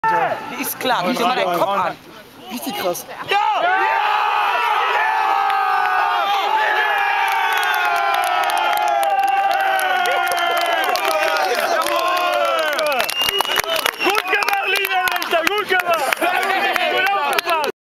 TSV Marl-Hüls - VfB Hüls 1:3 am Mittwoch, 21.09.2011 (1)
Kreispokalspiel. das 1:2 kurz vor Schluss